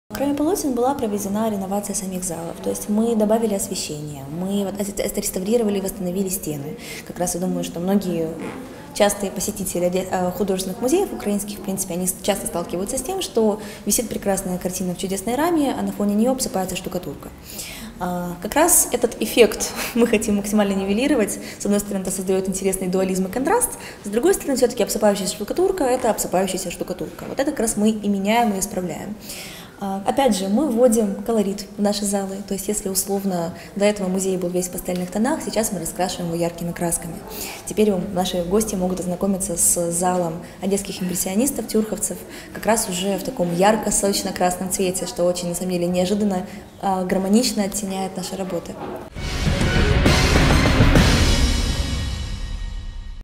Реэкспозиция и реновация залов Одесского худмузея
Анна Петрова, сотрудница Одесского художественного музея: «Кроме полотен была проведена реновация самих залов. Мы добавили освещения. Мы отреставрировали и восстановили стены. Частые посетители сталкиваются с тем. Что висит прекрасная картина в красивой раме, а над ней осыпается штукатурка. Как раз этот эффект мы хотим максимально нивелировать. С одной стороны, это создает интересный дуализм и контраст, с другой – обсыпающаяся штукатурка - это обсыпающаяся штукатурка. Опять же, мы вводим колорит в наши залы. Если раньше музей был в пастельных тонах, то сейчас раскрашиваем его яркими красками. Теперь наши гости могут ознакомиться с залом одесских импрессионистов уже в таком ярко-сочно-красном цвете. Что очень гармонично оттеняет наши работы».